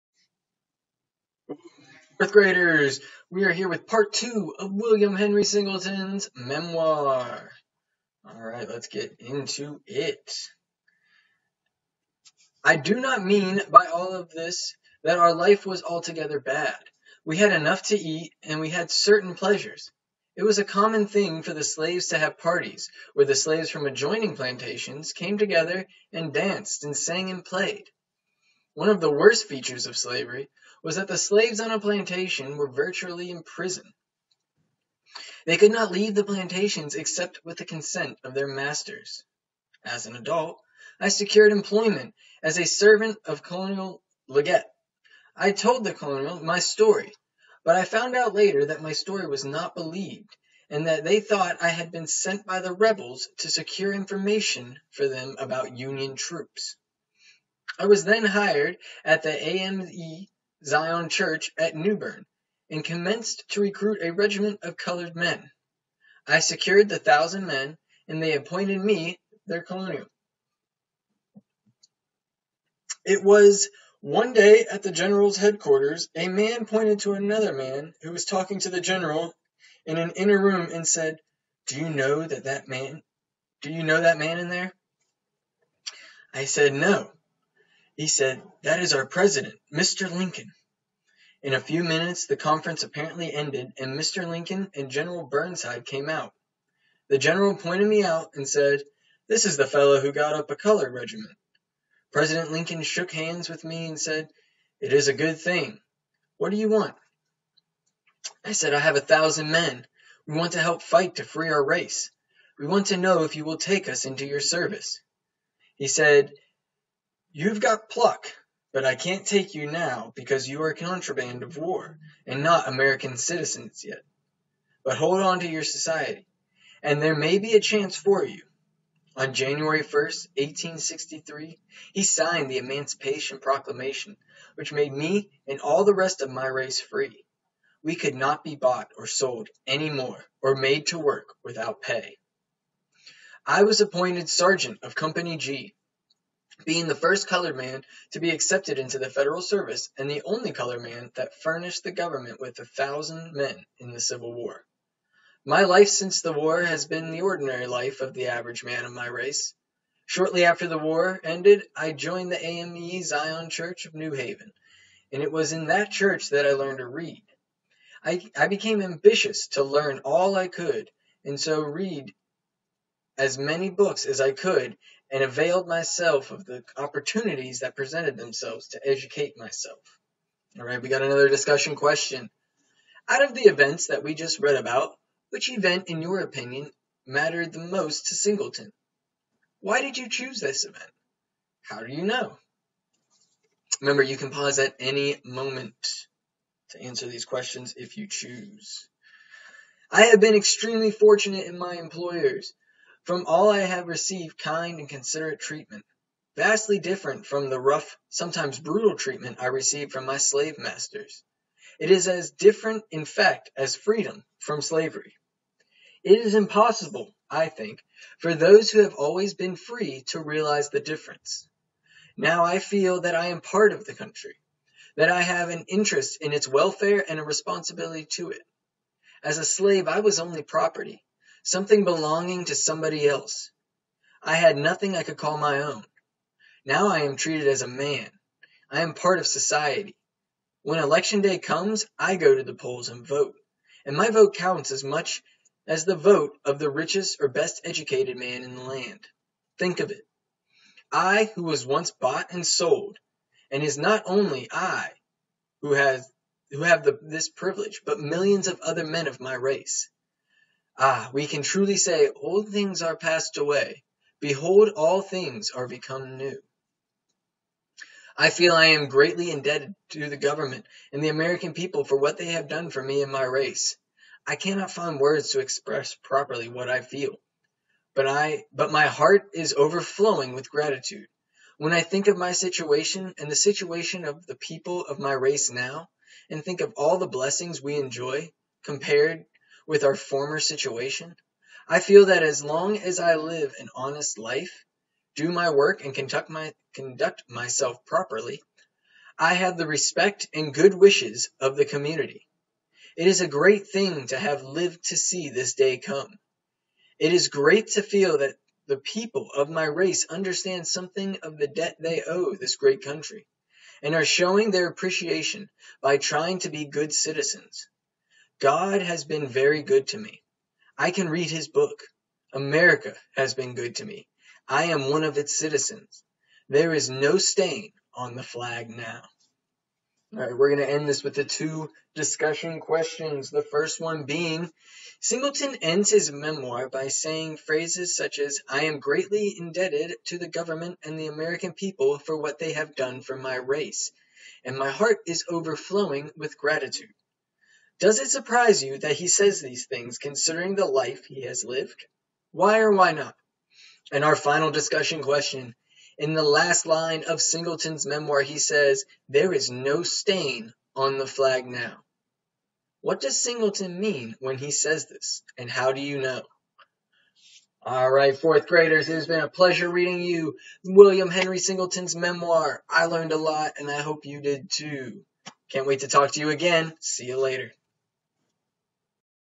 William Henry Singleton pt 2
Today we finish William Henry Singleton's Memoir. I also want to apologize for the audio it is the best I could do at the time.